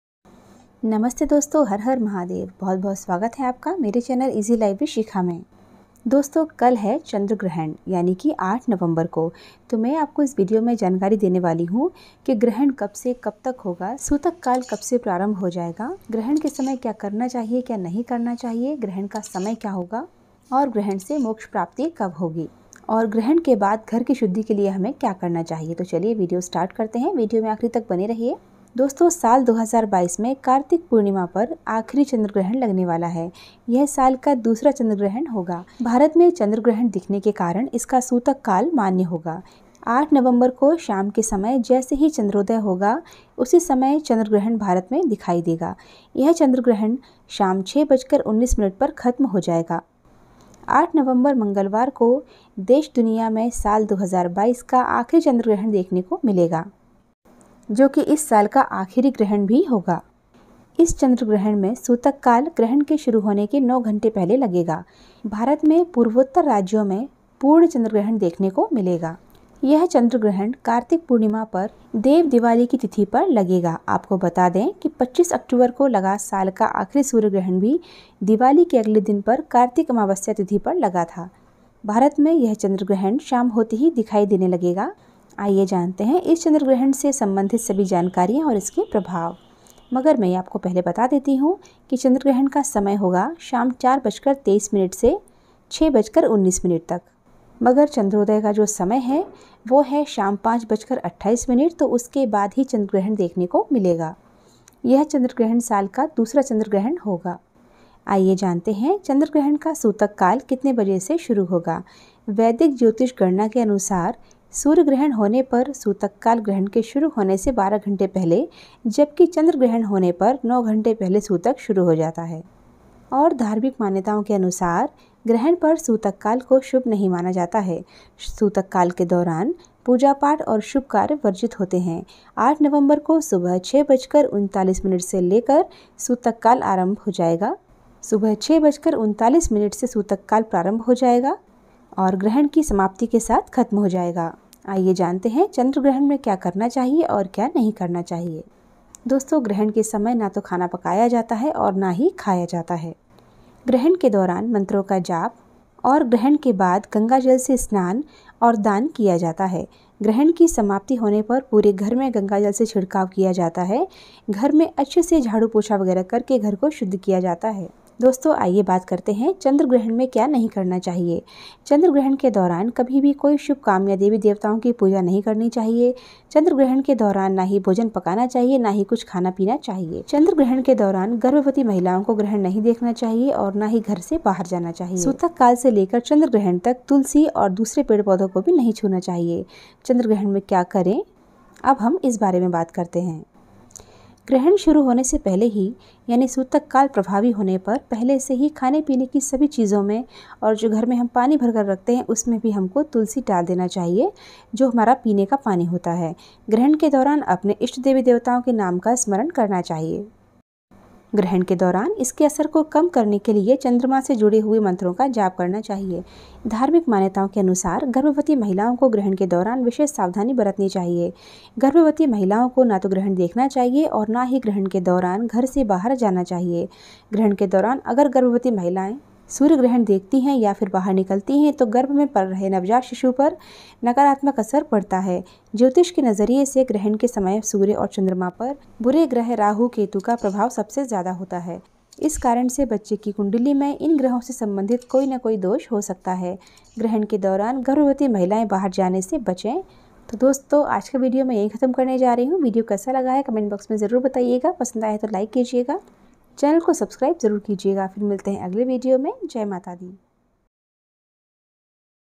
8 नवम्बर 2022 चंद्र ग्रहण का समय सूतक कल कब लगेगा गर्भवती महिलाएं क्या सावधानियां रखें
8 November 2022 chanद्र ग्रहण का समय सूतक कल कब लगेगा ग्रहण के दौरान क्या करें क्या न करें गर्भवती महिलाएं क्या सावधानियां रखें मोक्ष का समय और ग्रहण के बाद क्या करें करें

easy life with shikha easylifewithshikha